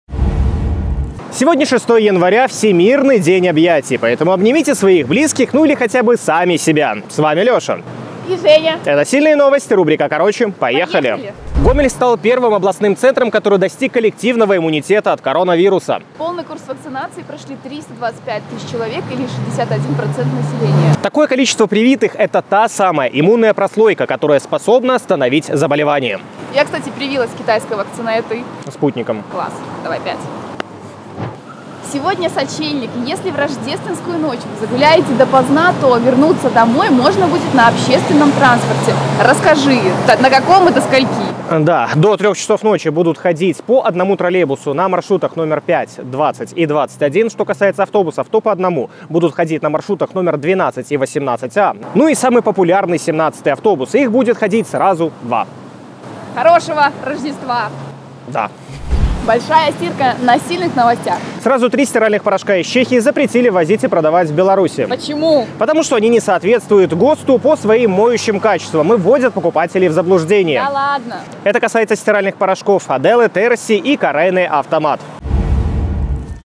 КОРОЧЕ: коллективный иммунитет в Гомеле, работа общественного транспорта, запрет стиральных порошков
Гомель первым из областных центров достиг коллективного иммунитета от коронавируса.

В Гомеле в рождественскую ночь будет ходить дополнительный транспорт.

В Беларуси запретят ввоз и продажу сразу трех стиральных порошков их Чехии.